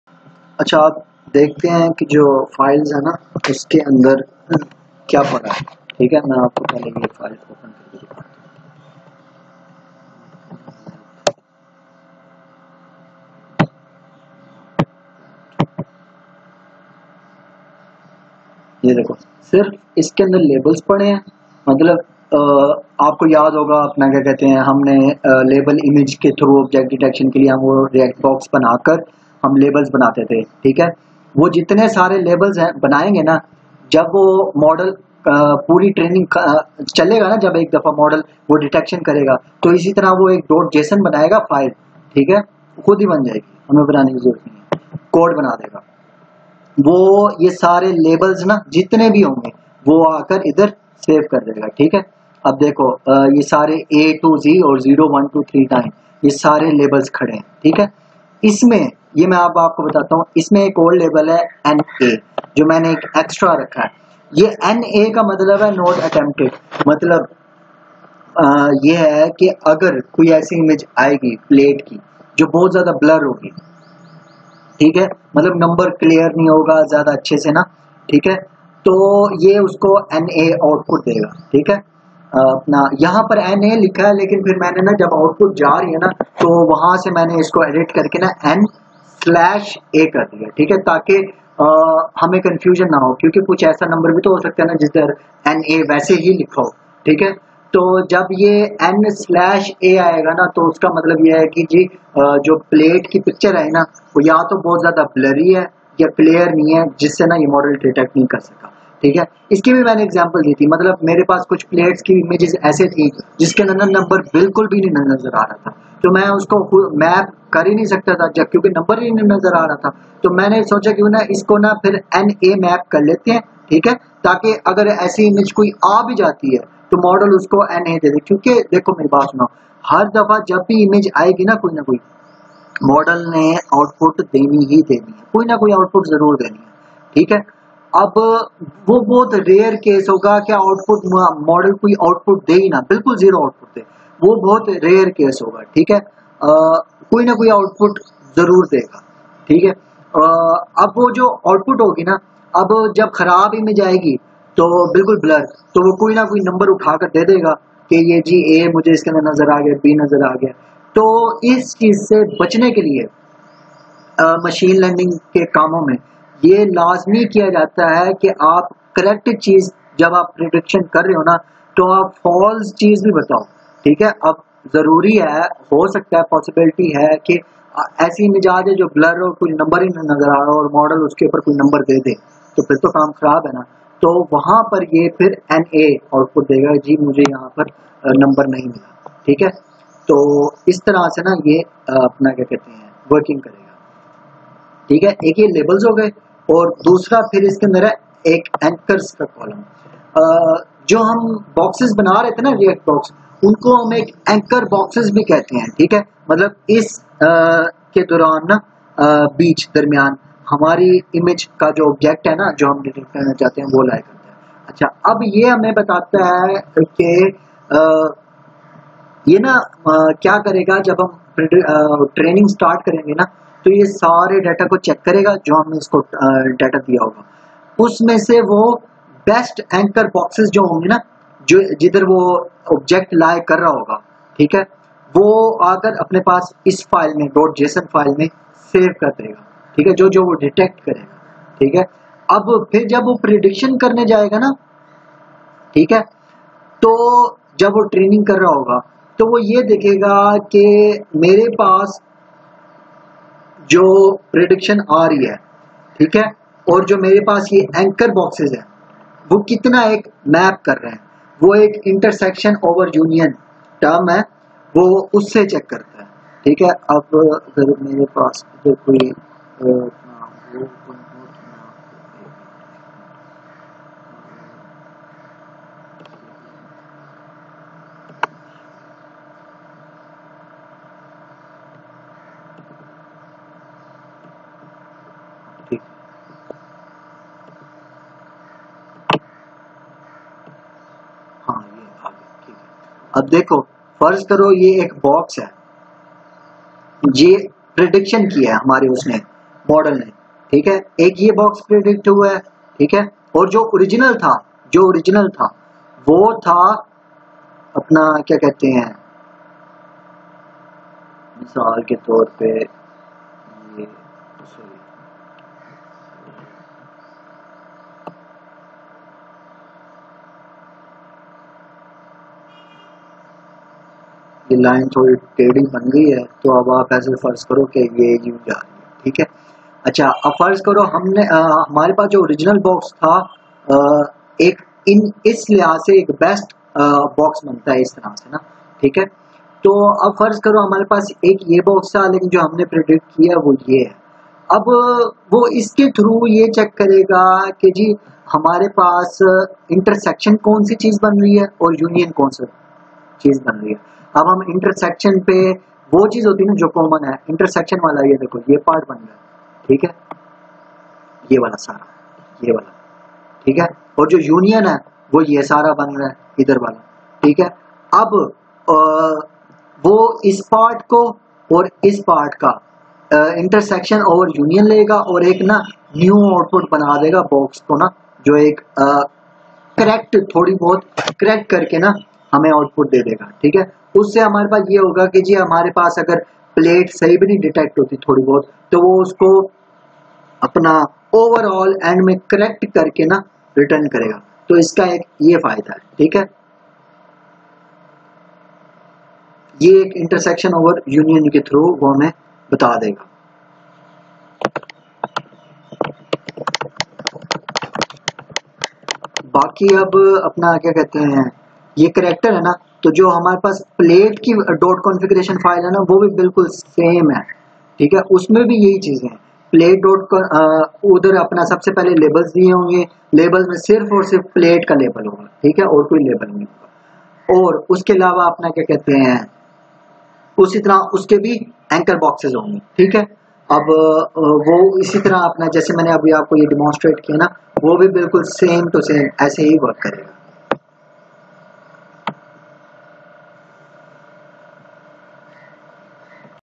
lecture # 6 Labels prediction | make Label in python | How to make Characters labels in python
License plate recognition system in python complete tutorial serious in this video we discusse about labels in license plate
if you want to learn complete python course then please visit our complete python course playlist